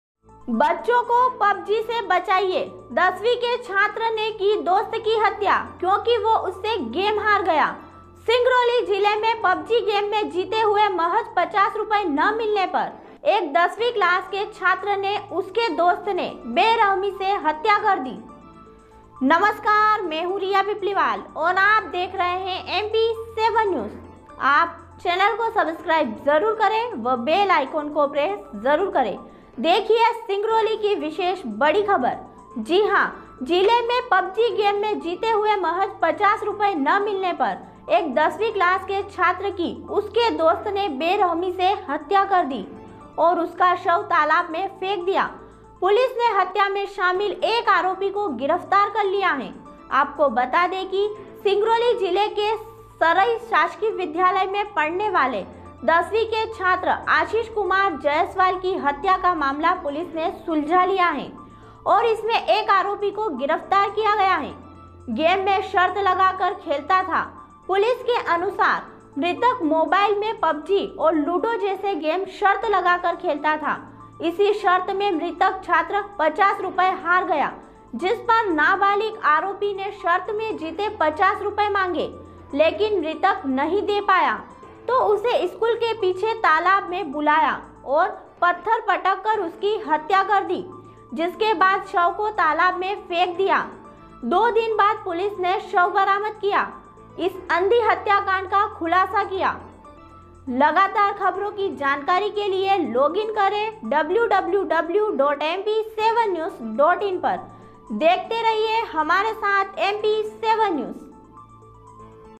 Pub G गेम के कारण दोस्त ने ली दोस्त की जान#pubGgame#game#mout#murder#mp7news#trending#breakingnews
@MP7NEWS pubg गेम के चलते दोस्त ने ली दोस्त की जान

🤵🏼‍♂Gopaldas Bairagi
Chief Editor & Journalist - MP 7 NEWS